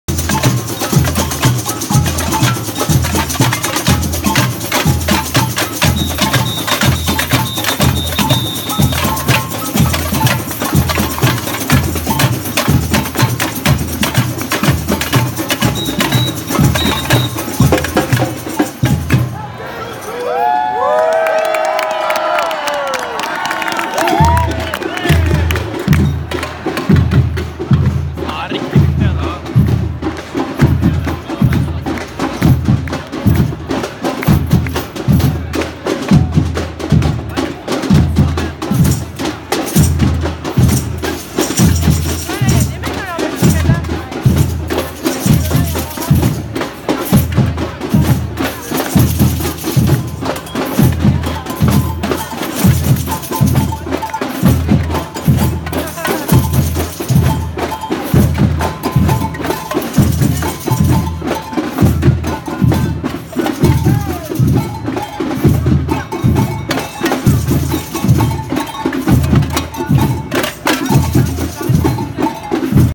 Grève du 5 décembre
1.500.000 personnes dans la rue sur l’ensemble du territoire, selon le la CGT.